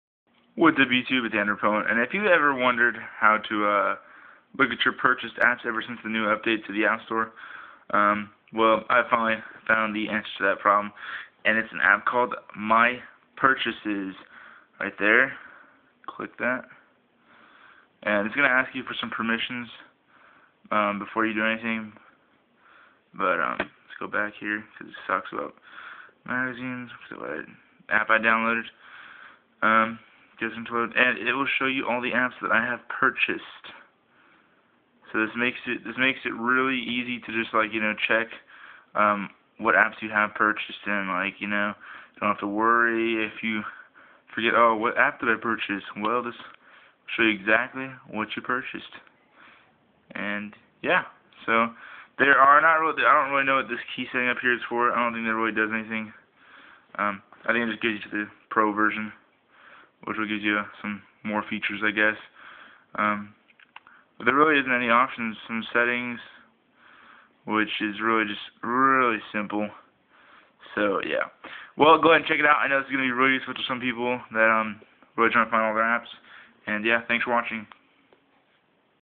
How to find your purchased apps on Android
This is the easiest way to find your purchased apps.

This video wad recorded with my captivate.